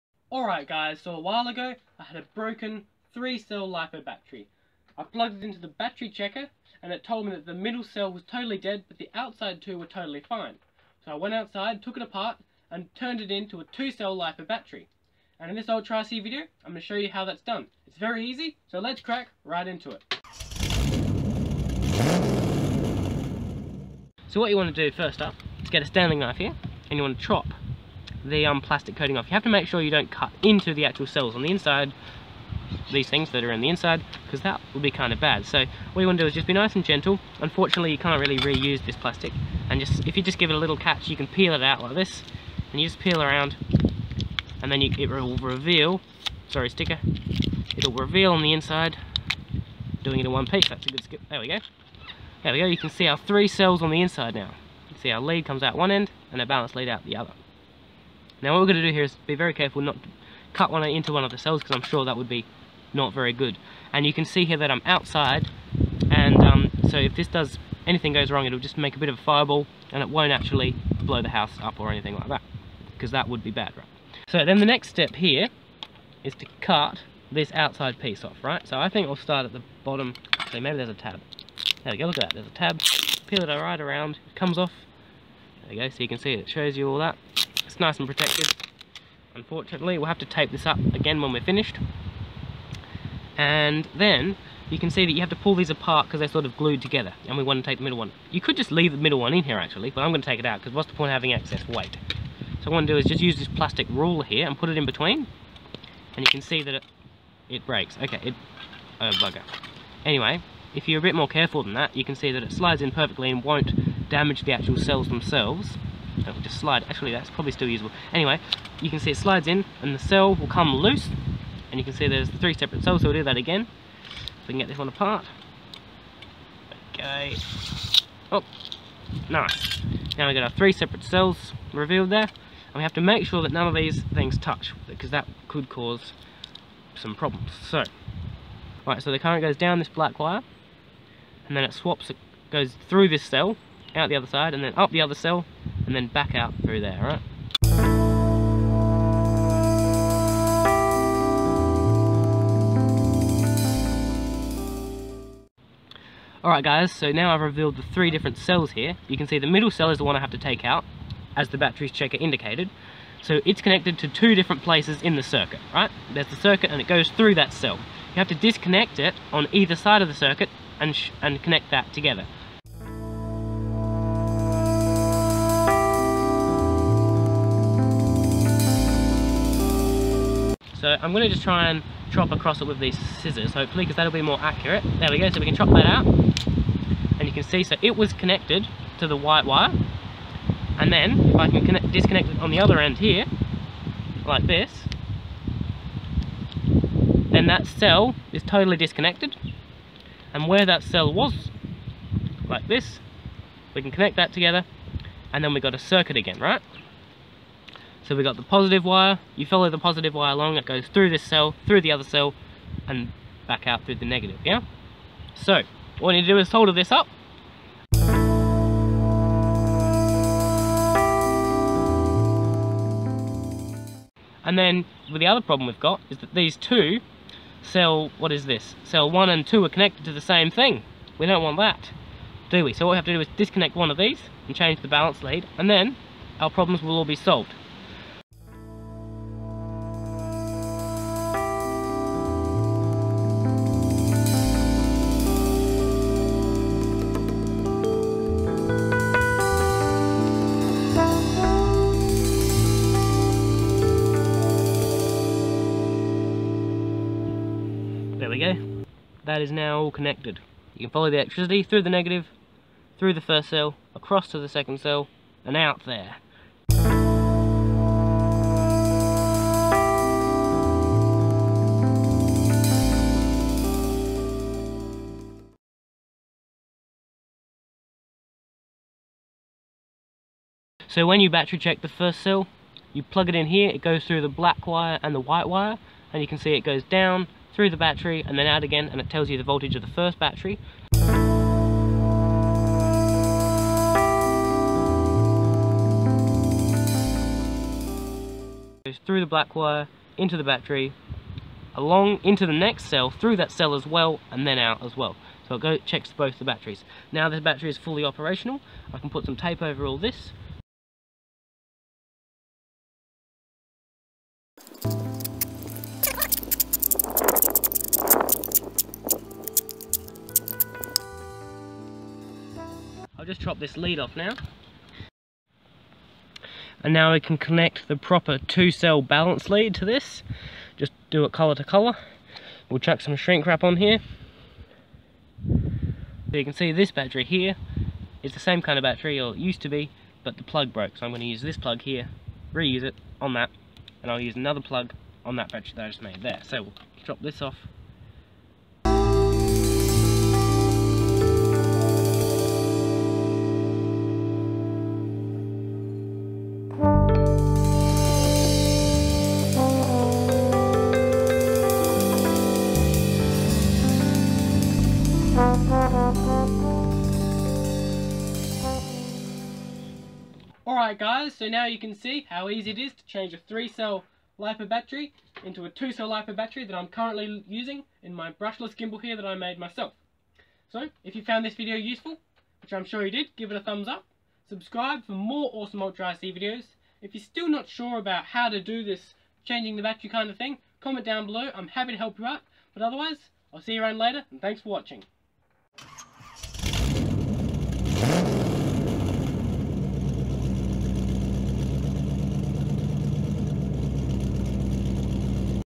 How to remove a damaged cell from a Lipo battery //ULTRA RC
I had a dead lipo battery and went out to fx it.
It was super easy and i wanted to share it with you so you can try it one day!

BY FOR NOW!!!